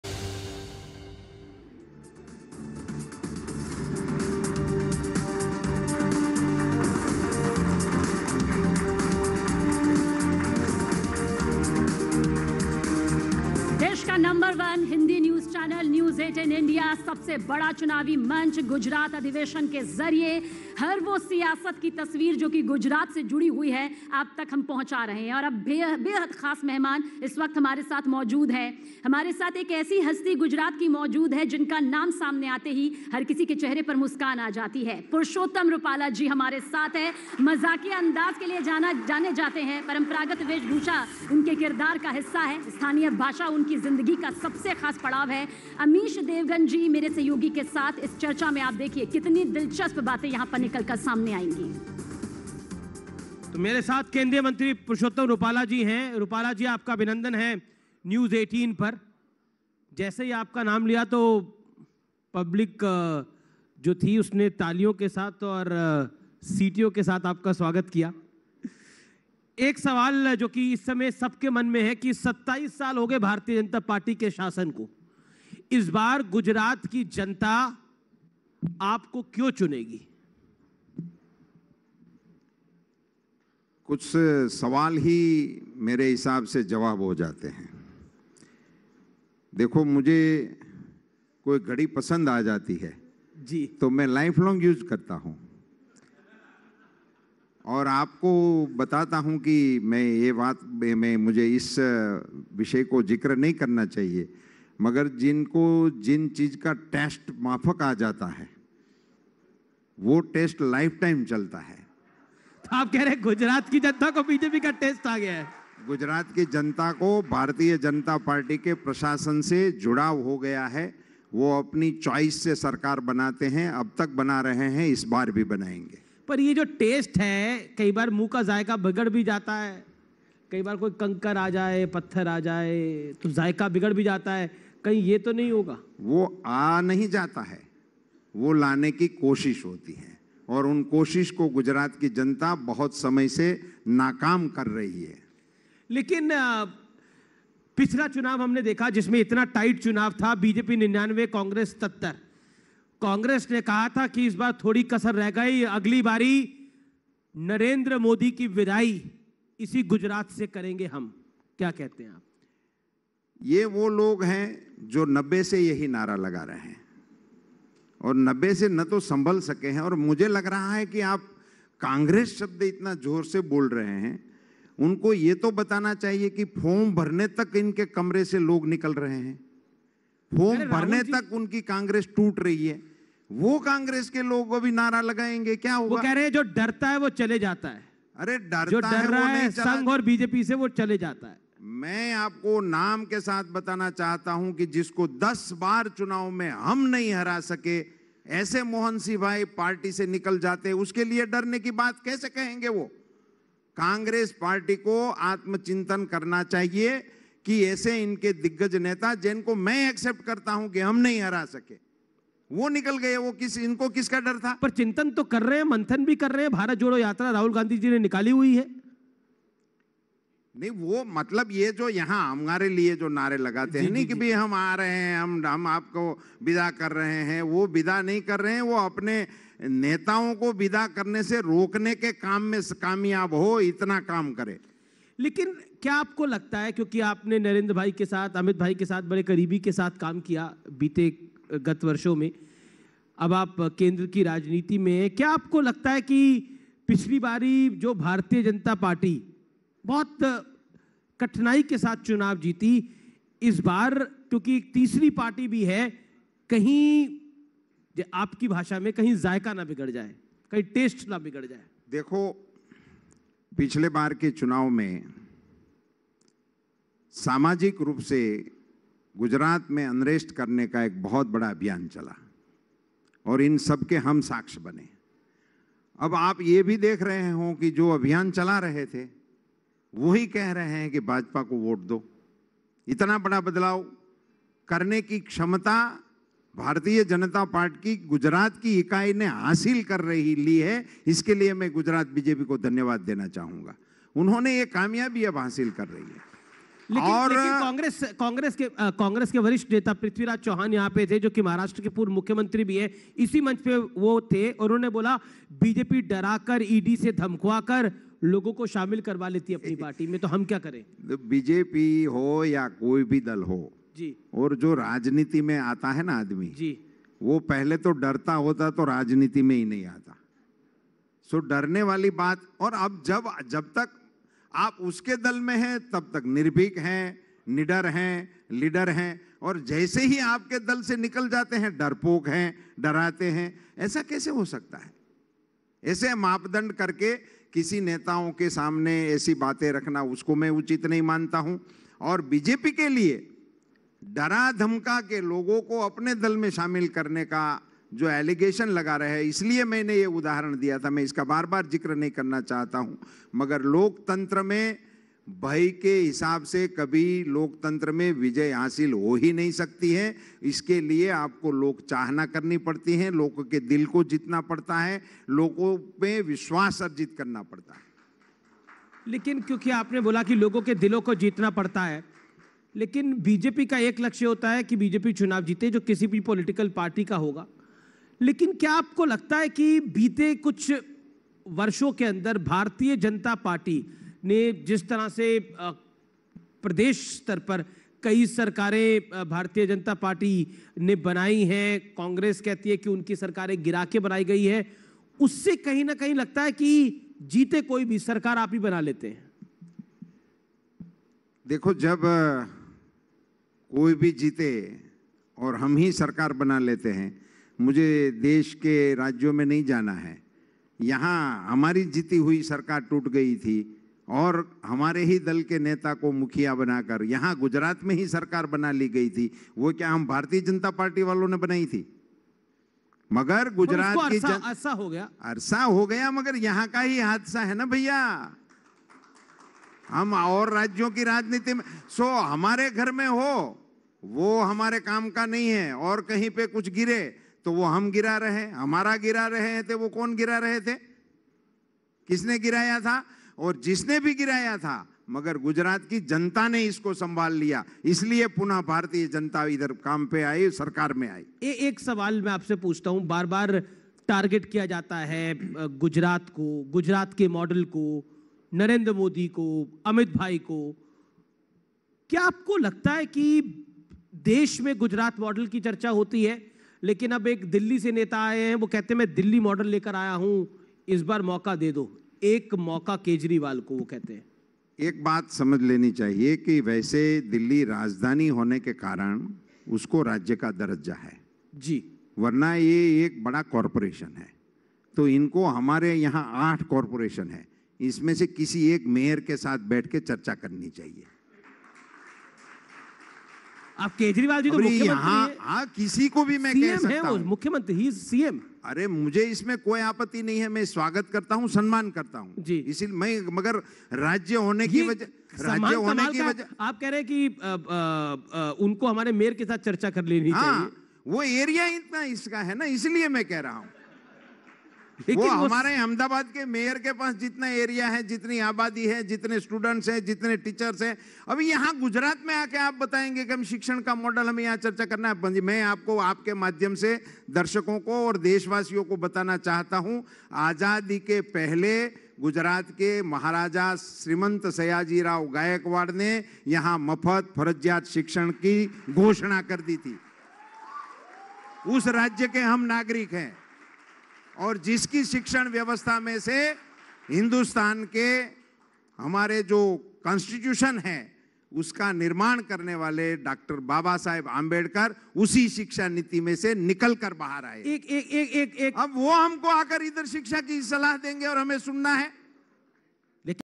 Gujarat Adhiveshan 2022 : गुजरात अधिवेशन में बोले Parshottam Rupala- जनता को BJP से जुड़ाव हो गया
Gujarat Adhiveshan 2022 : News18 India के Gujarat अधिवेशन में बीजेपी नेता और केंद्रीय मंत्री पुरुषोत्तम रुपाला (Parshottam Rupala) ने विधानसभा चुनाव में जीत का दावा करते हुए कहा कि ‘गुजरात की जनता को बीजेपी के प्रशासन से जुड़ाव हो गया है. अब तक सरकार बनाते आ रही हैं इस बार भी बनाएगी.’